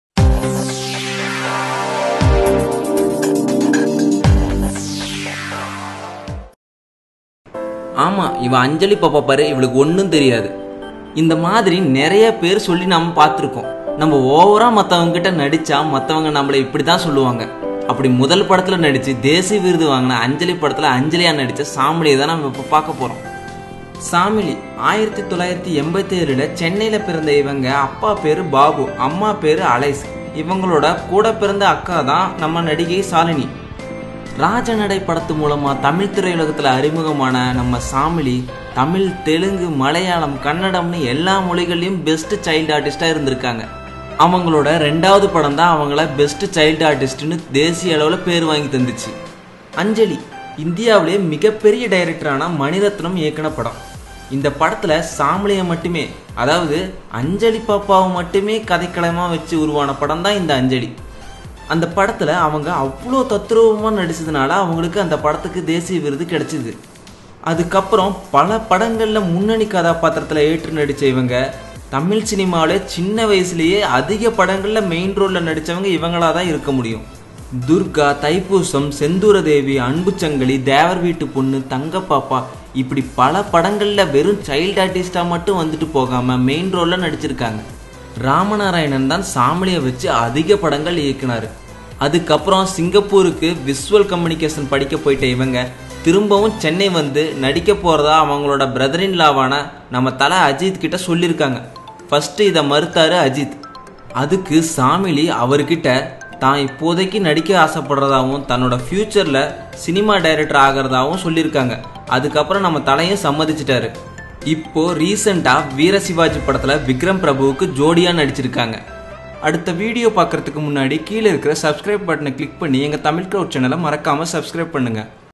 அஞ்சலி பாப்பா தற்போதைய நிலை | Tamil Cinema News | Kollywood News | Tamil Cinema Seithigal
Shamili is a beautiful actress who acted in tamil, telugu and Malayalam movies. She is better known as “Baby Shamili”. She is the little girl who acted in Mani Ratnam’s Anjali movie.

Welcome to the Tamil Crowd!